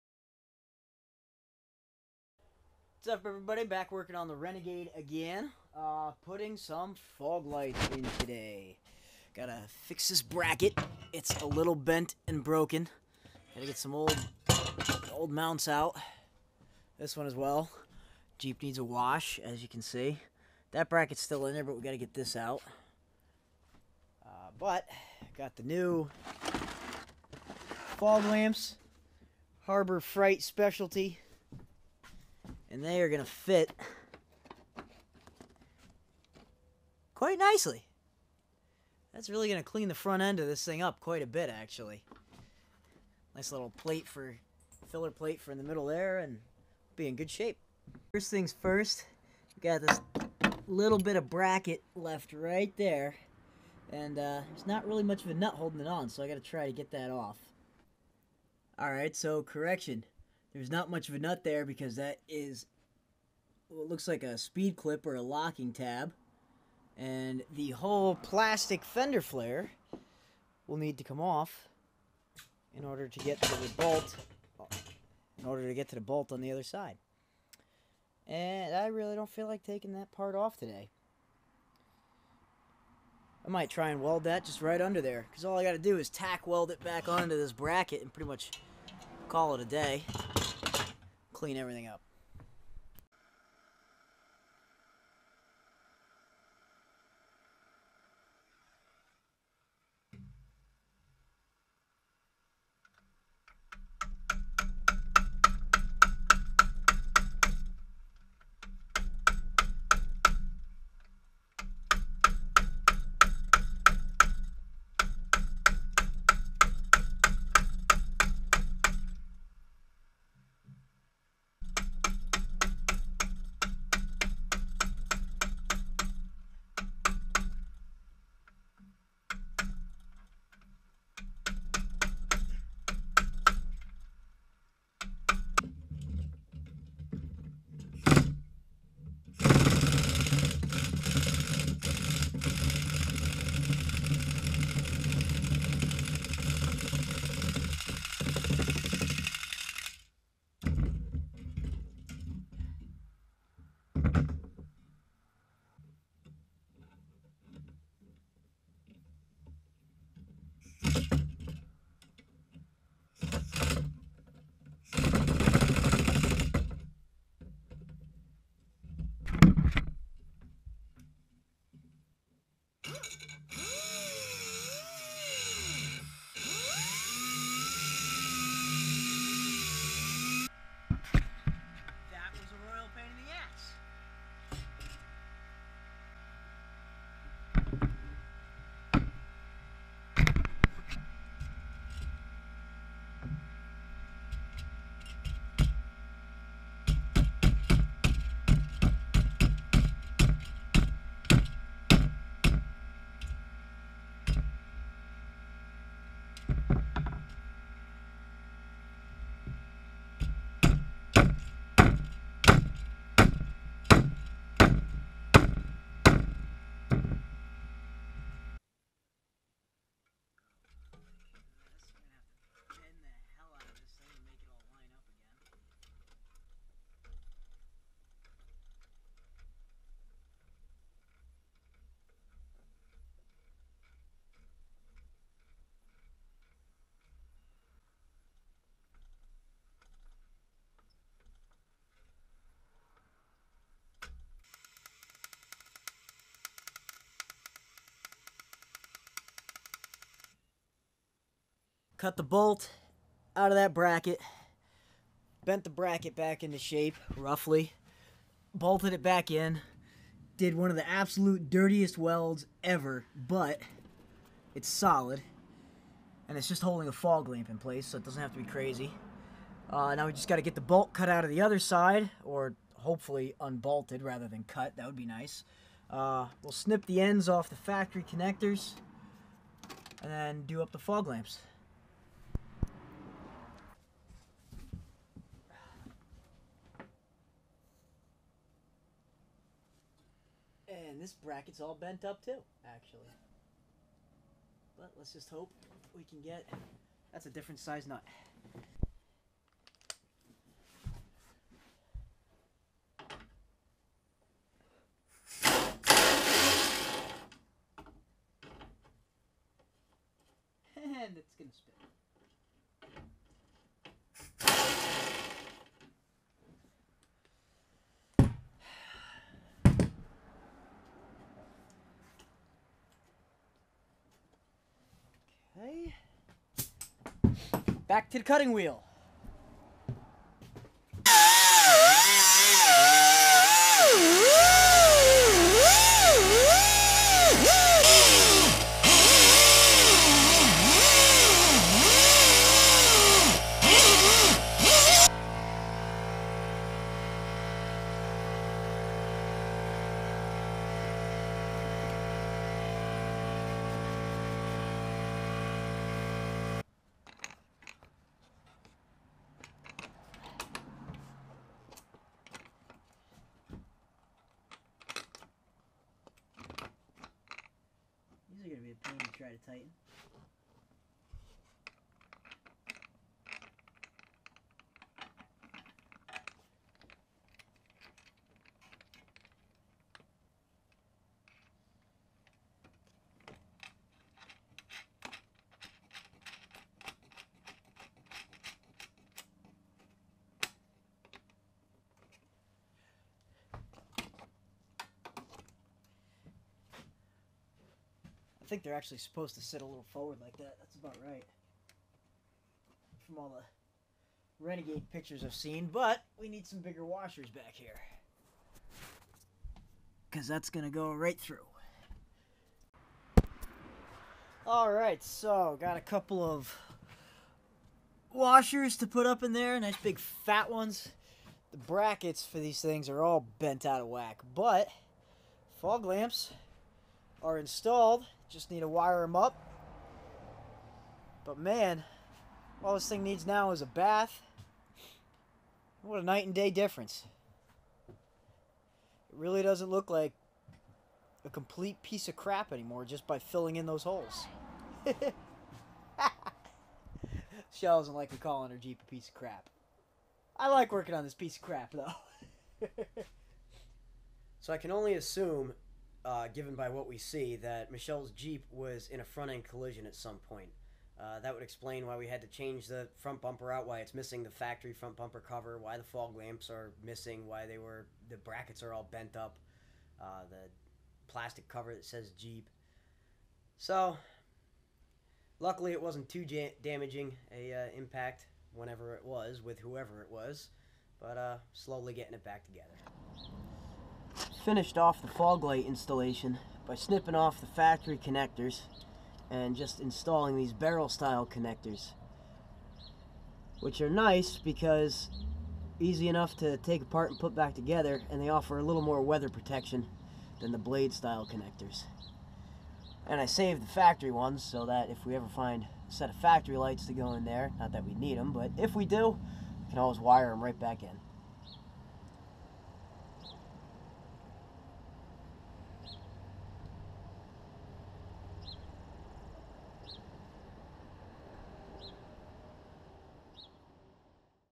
Rusty Renegade: Ep 08- Fog Lamp Install
Found a cheap set of fog lamps to fill the holes in the fenders. Looks like Chele's Jeep was in an accident at some point, the brackets for the fog lamps were all bent and broken. That would explain all the missing front end pieces.